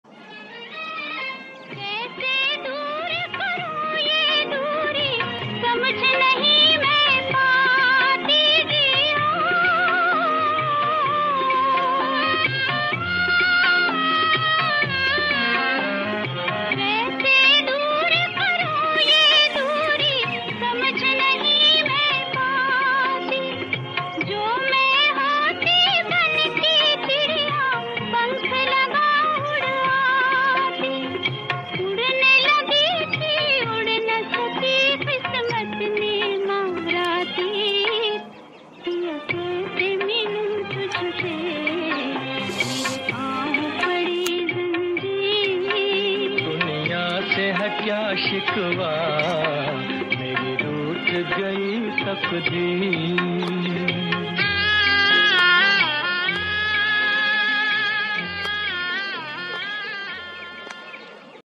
Broken wing
A poor Greater Flamingo with broken wing at Chandlai lake, Shivdaspura, Rajasthan
25012022